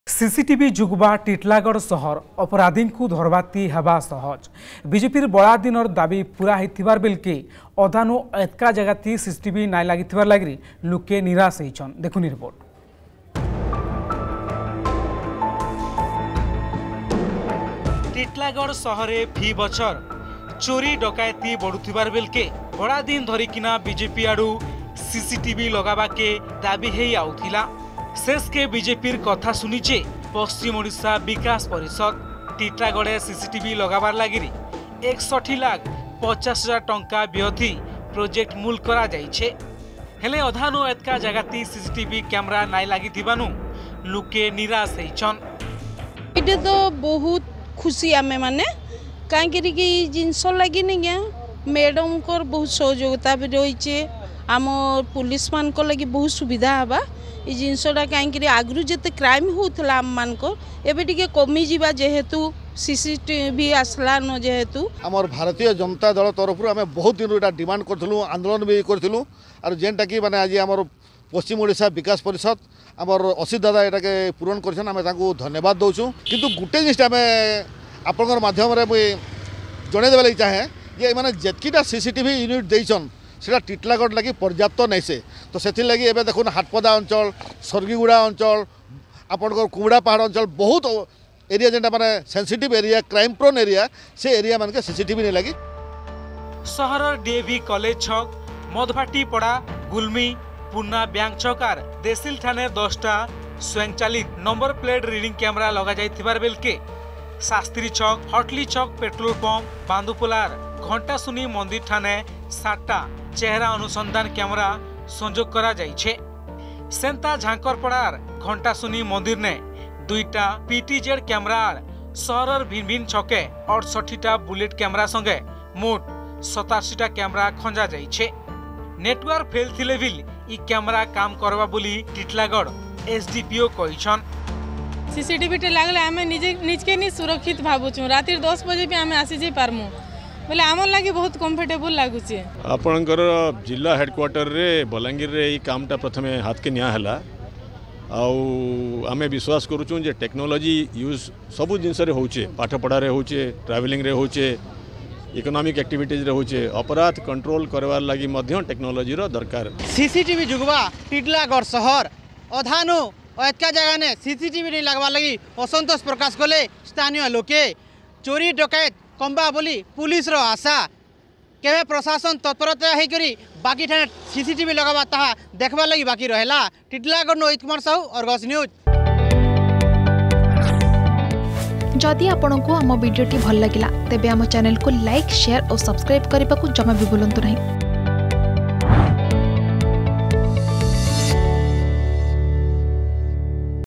ସିସି ଟିଭି ଯୁଗ୍‌ବ ଟିଟିଲାଗଡ଼ ସହର୍‌ || CCTV || Titilagarh
ସିସି ଟିଭି ଯୁଗ୍‌ବ ଟିଟିଲାଗଡ଼ ସହର୍‌ । ଅପରାଧୀକୁଁ ଧରବାଥି ହେବା ସହଜ୍‌ ।  ବିଜେପିର୍ ବଡା ଦିନର୍ ଦାବି ପୂରା ହେଇଥିବାର୍ ବେଲକେ । ଅଧାନୁ ଅଏଧକା ଜାଗାଥି ସିସି ଟିଭି ନାଇଁ ଲାଗିଥିବାର୍ ଲାଗିର୍ ଲୁକେ ନିରାଶ ହେଇଛନ୍ ।